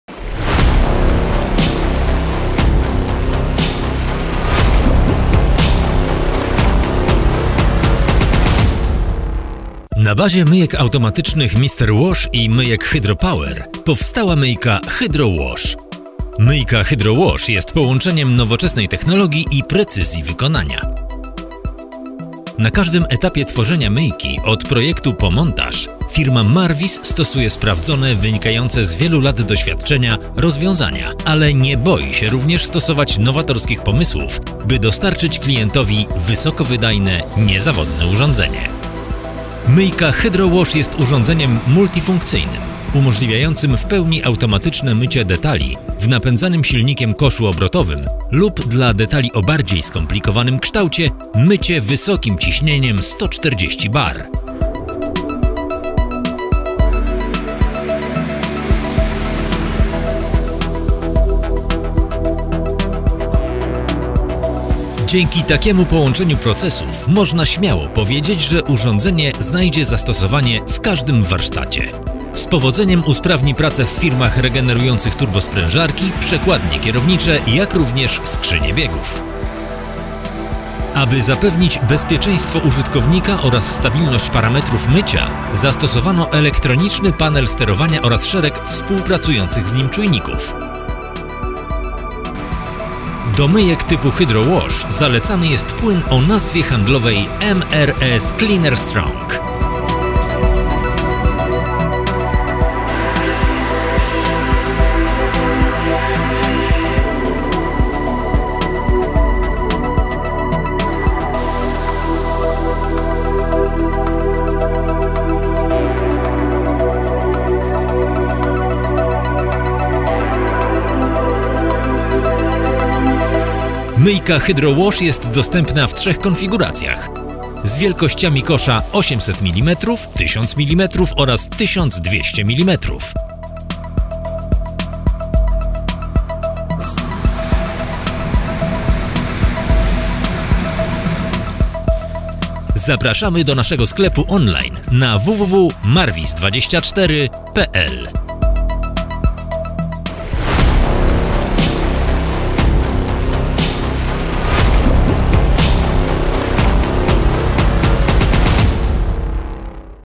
Myjka automatyczna Hydro Wash 2w1
HYDRO  WASH  jest połączeniem nowoczesnej technologii i precyzji wykonania. Na każdym etapie tworzenia myjki -od projektu po montaż- firma MARWIS stosuje sprawdzone, wynikające z wielu lat doświadczenia rozwiązania, ale nie boi się również stosować nowatorskich pomysłów, by dostarczyć klientowi wysokowydajne, niezawodne urządzanie. Myjka Hydro-Wash jest urządzeniem multifunkcyjnym, umożliwiającym w pełni automatyczne mycie detali w napędzanym silnikiem koszu obrotowym, lub dla detali o bardziej skomplikowanym kształcie mycie wysokim ciśnieniem 140bar. Dzięki takiemu połączeniu procesów, można śmiało powiedzieć, że urządzenie znajdzie zastosowanie w każdym warsztacie. Z powodzeniem usprawni pracę w firmie regenerującym turbosprężarki, przekładnie kierownicze jak również skrzynie biegów. Aby zapewnić bezpieczeństwo użytkownika, oraz stabilność parametrów mycia zastosowano elektroniczny panel sterowania oraz szereg współpracujących z nim czujników.

STANDARDOWE WYPOSAŻENIE MYJKI:

Czujnik rozłączenia pokrywy domykającej podczas otwierania jej w trakcie procesu mycia,
Czujnik poziomy cieczy w zbiorniku, zabezpieczający przed pracą na sucho,
Czujnik temperatury cieczy,
Ramiona natryskowe regulowane, z dyszami ze stali nierdzewnej renomowanej firmy Spraying System
Kosz mechanicznie obrotowy,
Pompa wysokiego ciśnienia 140bar
Pistolet sprężonego powietrza
Reduktor ciśnienia wraz zanieczyszczeń odwadniaczem
Filtr zanieczyszczeń nierozpuszczalnych zgrubny (filtr siatkowy oczko 0,8mm)
Filtr zanieczyszczeń dokładny
Stopki regulowane
Zawór spustowy,

Gwarancja 12 miesięcy

Myjki HYDRO WASH spełniają normy bezpieczeństwa oraz jakości CE